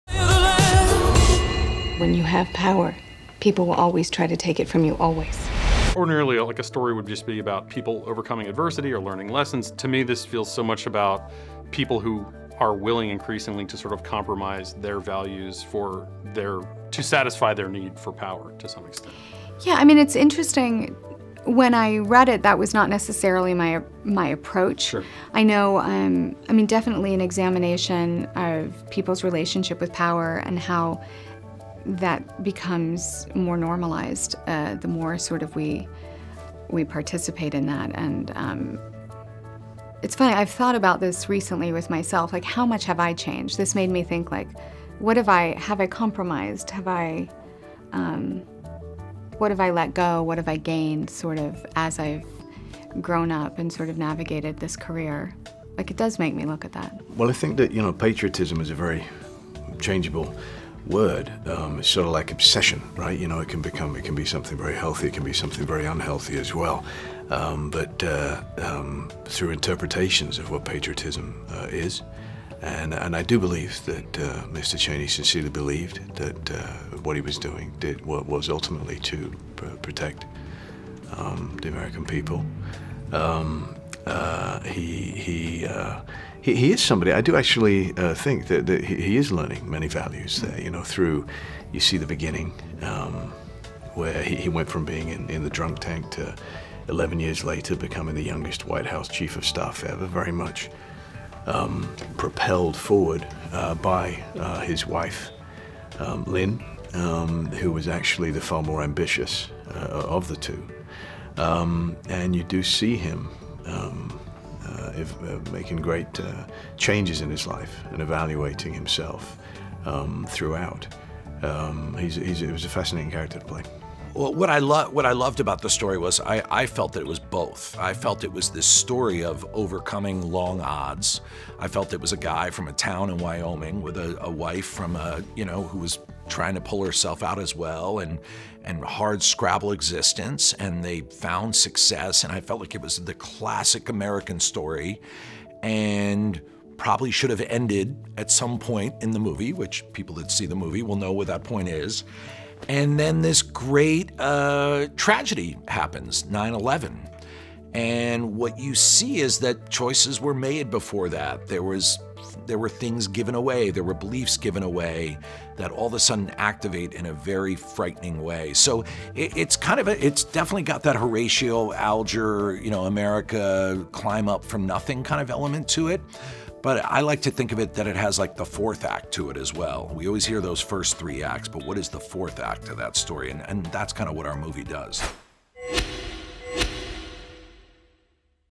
'Vice' Creators on Sacrificing Values for the American Dream
"Vice" doesn't follow a traditional narrative, in that the subjects tend to askew values in an effort to gain power. Director Adam McKay along with actors Christian Bale and Amy Adams discuss this dynamic and how it uniquely applies to this story.

"Vice" comes to theaters Christmas Day.

Plot Summary
VICE explores the epic story about how a bureaucratic Washington insider quietly became the most powerful man in the world as Vice-President to George W. Bush, reshaping the country and the globe in ways that we still feel today.